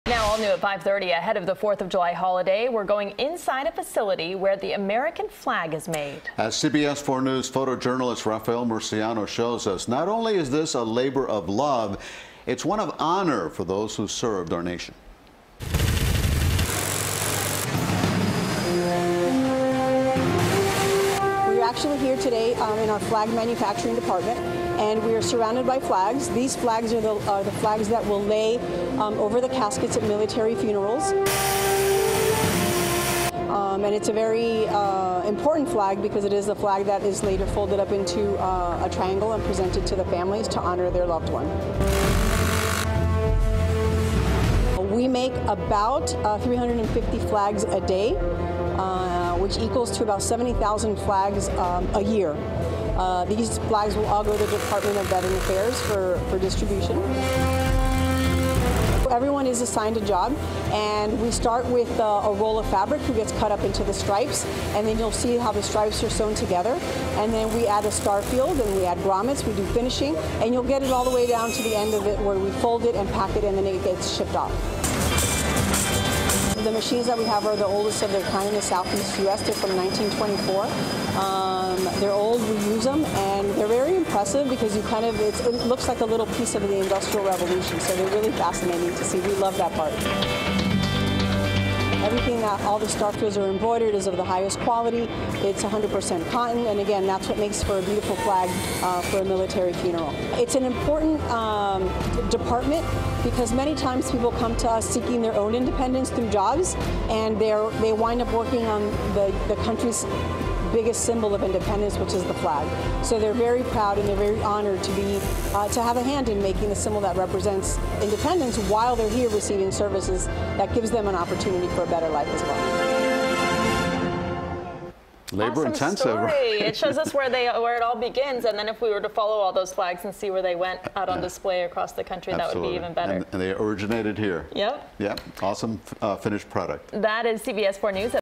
Inside Look At Facility That Makes American Flags
Ahead of the Fourth of July later this week, CBS4 got an inside look at a facility where the American flag is made.